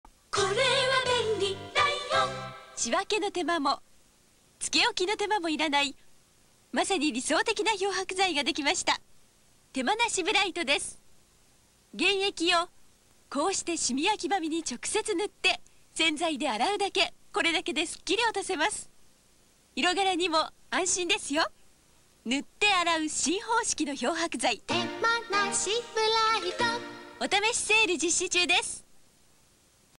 ライオン 手間なしブライト CM30秒
寺田理恵子さん出演　新発売時の物